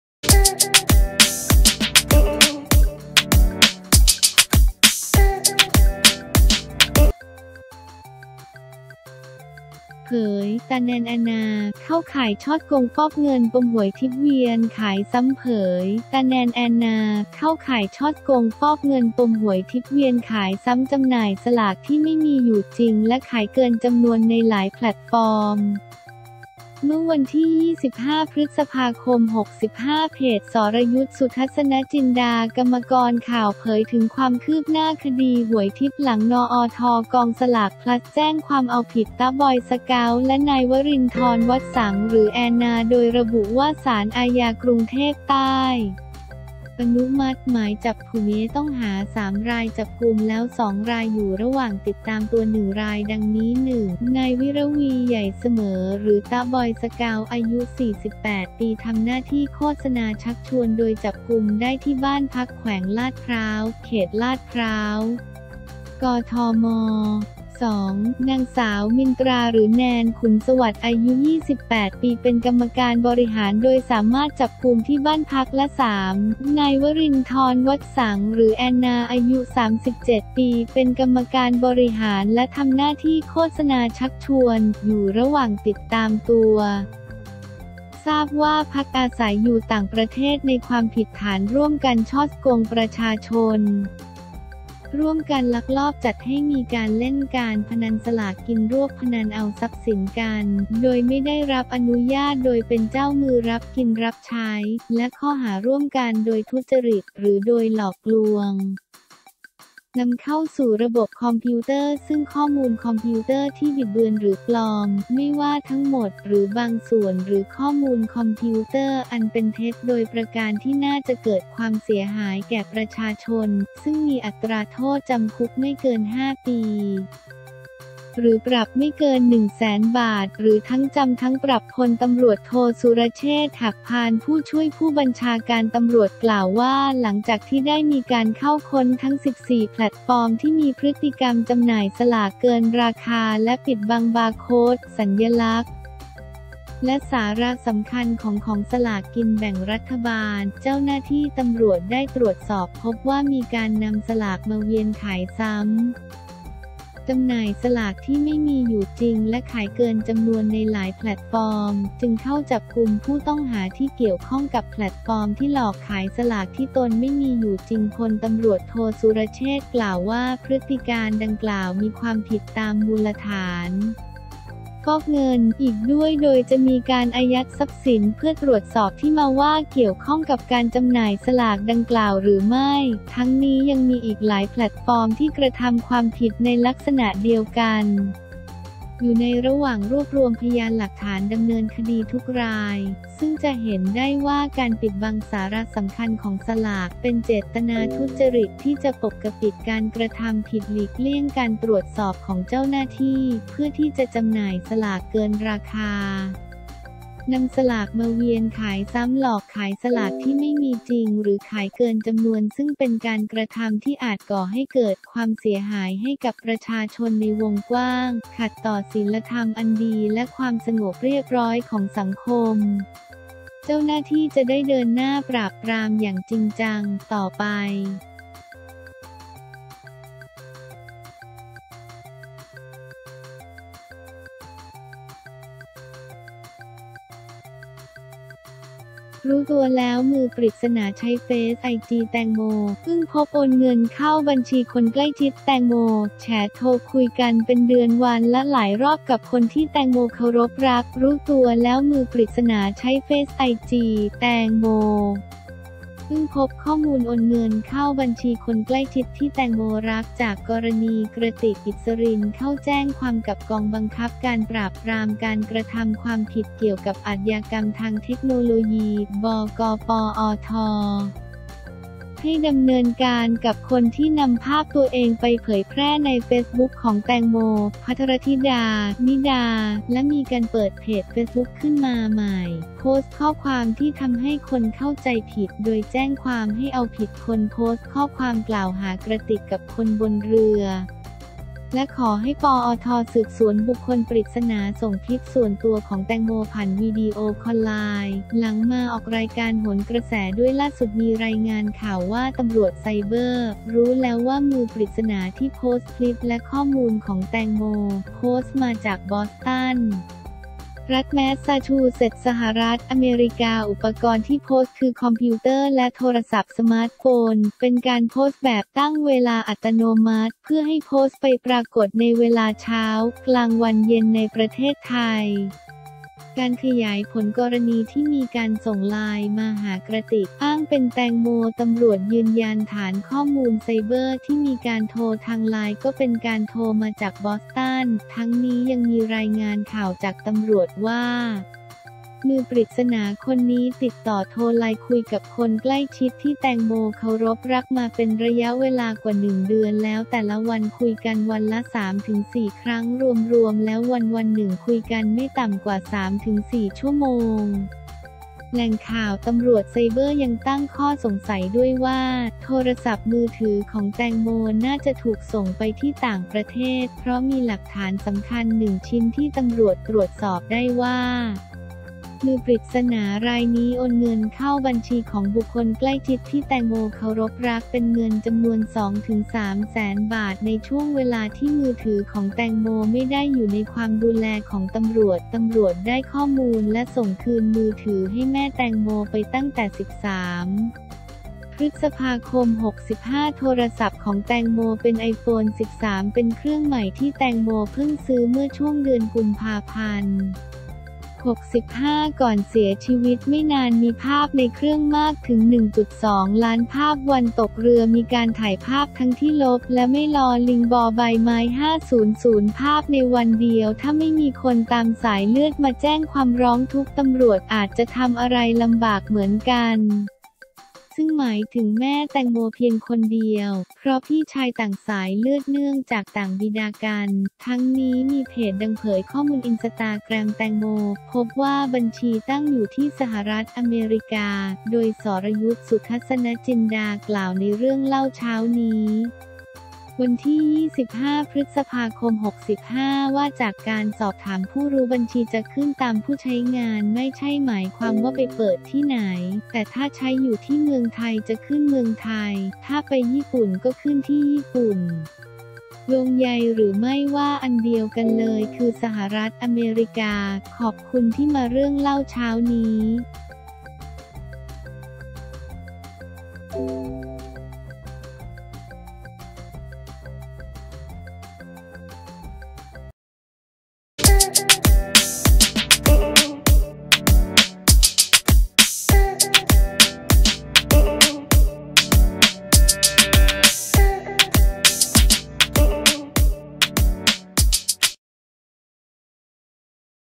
เผย 'ต๊ะ แนน แอนนา' เข้าข่ายฉ้อโกง ฟอกเงิน ปมหวยทิพย์ เวียนขายซ้ำ
เผย ‘ต๊ะ-แนน-แอนนา’ เข้าข่ายฉ้อโกง-ฟอกเงิน ปมหวยทิพย์-เวียนขายซ้ำ จำหน่ายสลากที่ไม่มีอยู่จริง และขายเกินจำนวนในหลายแพลตฟอร์ม
เมื่อวันที่ 25 พ.ค.65 เพจ สรยุทธ สุทัศนะจินดา กรรมกรข่าว เผยถึงความคืบหน้าคดีหวยทิพย์ หลัง นอท กองสลากพลัส แจ้งความเอาผิดต๊ะ บอยสเกาท์ และนายวรินทร วัตรสังข์ หรือแอนนา
โดยระบุว่า ศาลอาญากรุงเทพใต้ อนุมัติหมายจับผุ้ต้องหา 3 ราย จับกุมแล้ว 2 ราย อยู่ระหว่างติดตามตัว 1 ราย ดังนี้
1.นายวินรวีร์ ใหญ่เสมอ หรือต๊ะ บอยสเกาท์ อายุ 48 ปี ทำหน้าที่โฆษณาชักชวน โดยจับกุมได้ที่บ้านพักแขวงลาดพร้าว เขตลาดพร้าว กทม. 2.น.ส.มินตรา หรือแนน ขุนสวัสดิ์ อายุ 28 ปี เป็นกรรมการบริหาร โดยสามารถจับกุมที่บ้านพัก และ3.นายวรินทร วัตรสังข์ หรือแอนนา อายุ 37 ปี เป็นกรรมการบริหารและทำหน้าที่โฆษณาชักชวน อยู่ระหว่างติดตามตัว ทราบว่าพักอาศัยอยู่ต่างประเทศ

ในความผิดฐาน ร่วมกันฉ้อโกงประชาชน, ร่วมกันลักลอบจัดให้มีการเล่นการพนันสลากกินรวบพนันเอาทรัพย์สินกัน โดยไม่ได้รับอนุญาตโดยเป็นเจ้ามือรับกินรับใช้ และข้อหาร่วมกันโดยทุจริต หรือโดยหลอกลวง นำเข้าสู่ระบบคอมพิวเตอร์ซึ่งข้อมูลคอมพิวเตอร์ที่บิดเบือนหรือปลอม ไม่ว่าทั้งหมดหรือบางส่วน หรือข้อมูลคอมพิวเตอร์อันเป็นเท็จโดยประการที่น่าจะเกิดความเสียหายแก่ประชาชน ซึ่งมีอัตราโทษจำคุกไม่เกิน 5 ปี หรือปรับไม่เกิน 1 แสนบาท หรือทั้งจำทั้งปรับ
พล.ต.ท.สุรเชษฐ์ หักพาล ผู้ช่วย ผบ.ตร.กล่าวว่า หลังจากที่ได้มีการเข้าค้นทั้ง 14 แพลตฟอร์ม ที่มีพฤติกรรมจำหน่ายสลากเกินราคาและปิดบังบาร์โค้ด สัญลักษณ์ และสาระสำคัญของของสลากกินแบ่งรัฐบาล เจ้าหน้าที่ตำรวจได้ตรวจสอบพบว่ามีการนำสลากมาเวียนขายซ้ำ , จำหน่ายสลากที่ไม่มีอยู่จริง และขายเกินจำนวนในหลายแพลตฟอร์ม จึงเข้าจับกุมผู้ต้องหาที่เกี่ยวข้องกับแพลตฟอร์มที่หลอกขายสลากที่ตนไม่มีอยู่จริง

พล.ต.ท.สุรเชษฐ์ กล่าวว่า พฤติการณ์ดังกล่าวมีความผิดตามมูลฐาน ‘ฟอกเงิน’ อีกด้วย โดยจะมีการอายัดทรัพย์สิน เพื่อตรวจสอบที่มาว่าเกี่ยวข้องกับการจำหน่ายสลากดังกล่าวหรือไม่